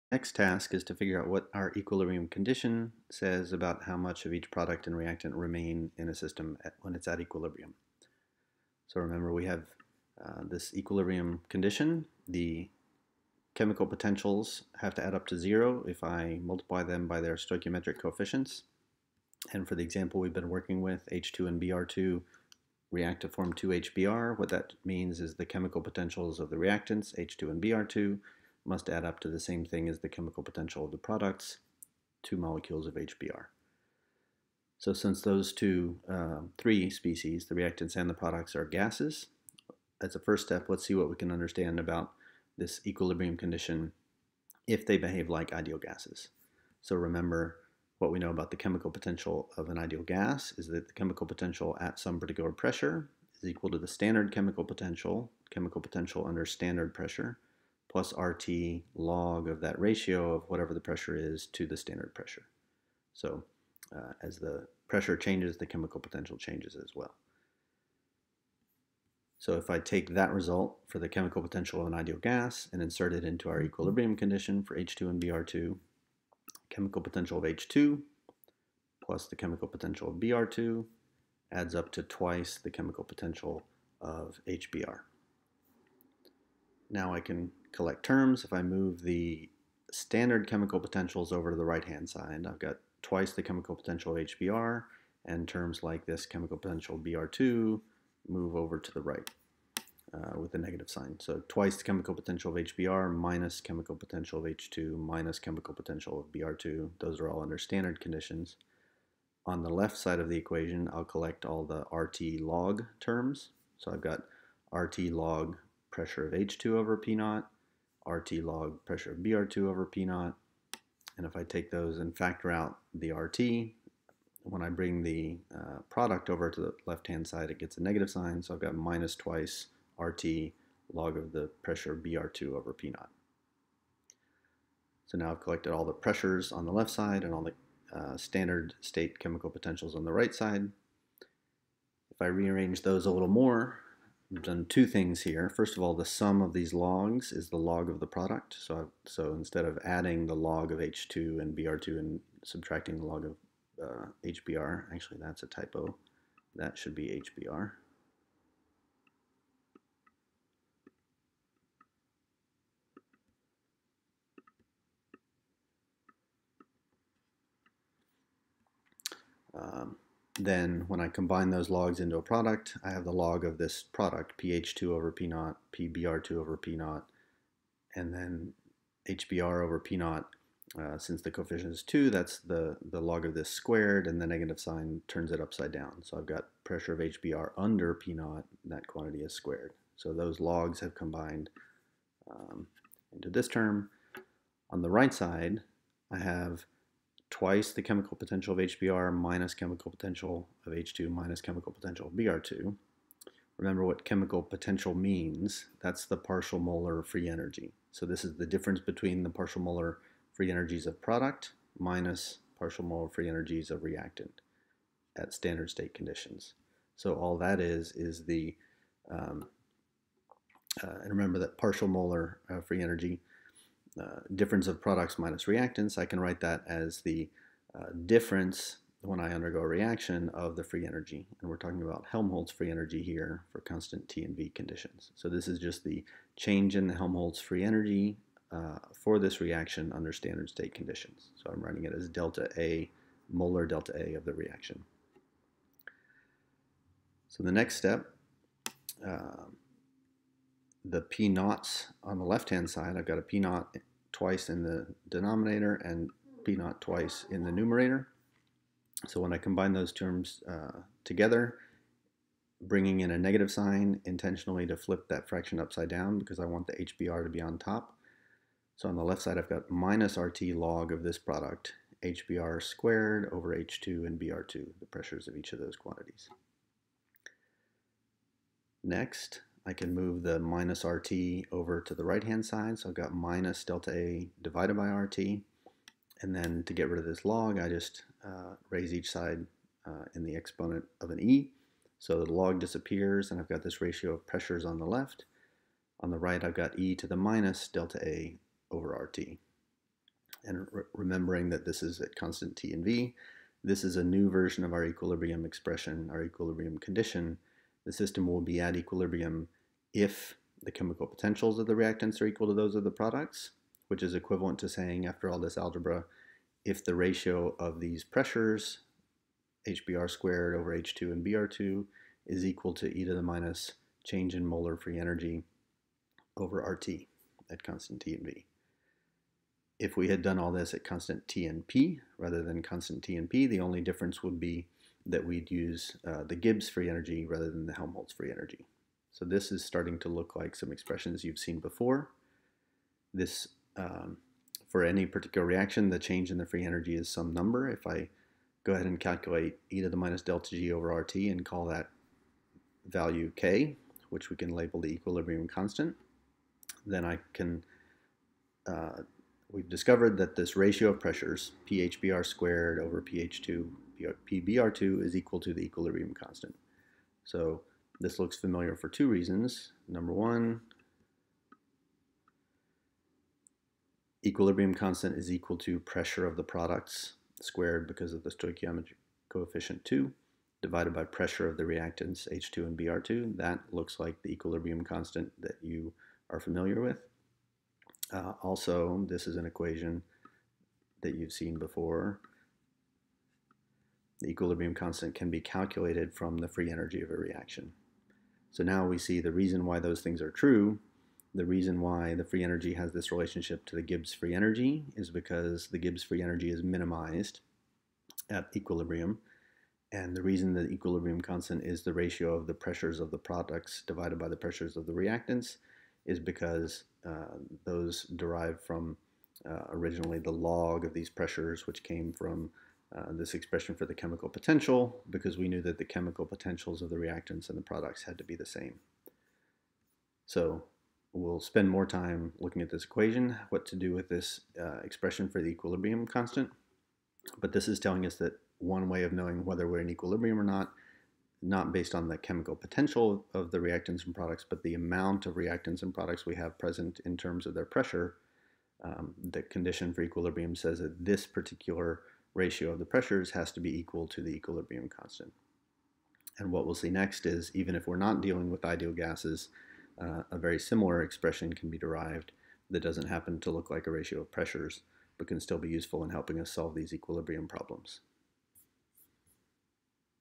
Equilibrium Constant (Ideal Gases)
This video lecture explores what must be true about ideal gases in chemical equilibrium with each other, which leads to the introduction of the equilibrium constant (K).